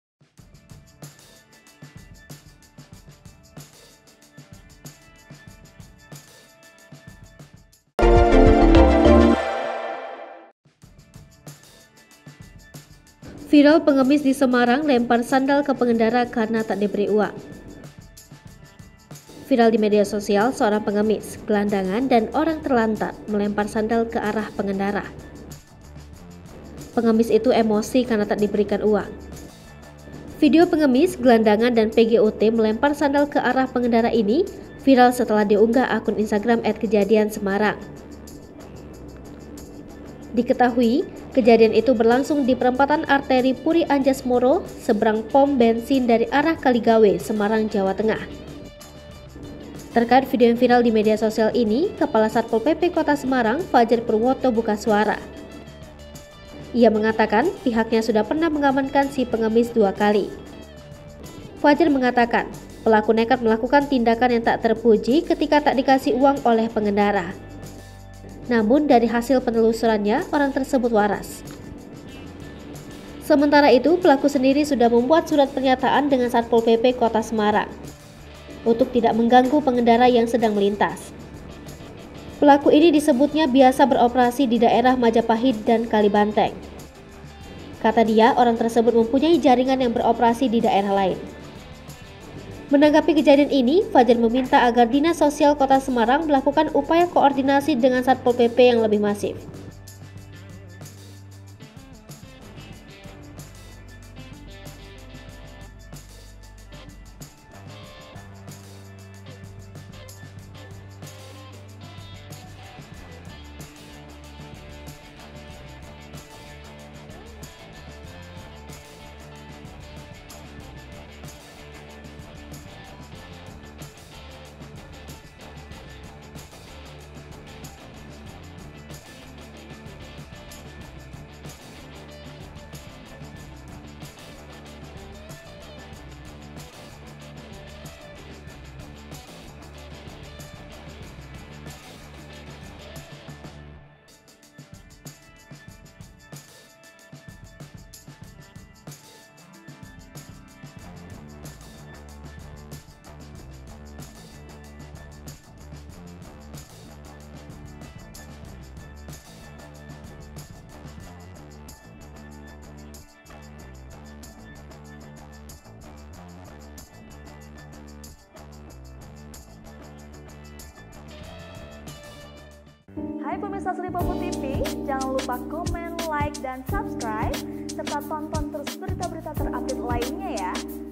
Tak Diberi Uang, Pengemis di Semarang Lempar Sandal ke Pengendara Viral
Pengemis itu emosi karena tak diberikan uang.

Video pengemis, gelandangan, dan PGOT melempar sandal ke arah pengendara ini viral setelah diunggah akun Instagram @kejadiansmg.

Berita Selengkapnya :
Yuk lihat informasi terkini bersama kami, di:

Palembang.tribunnews.com

Editor: Sarah
Sumber: Instagram @kejadiansmg